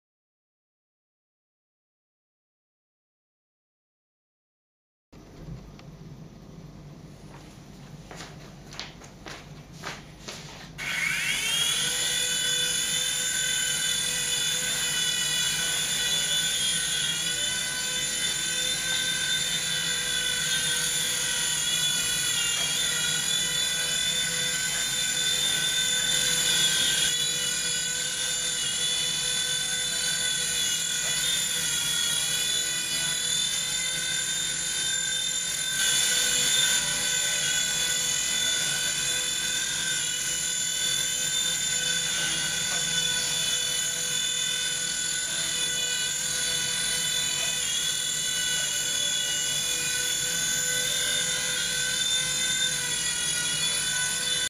My Falcon XVIII 8913 Helicopter From Ebay
The first helicopter that i had did not work. So i emailed the seller and he sent me a new one free of charge. Its actually a pretty good helicopter and pretty robust. XVIII Falcon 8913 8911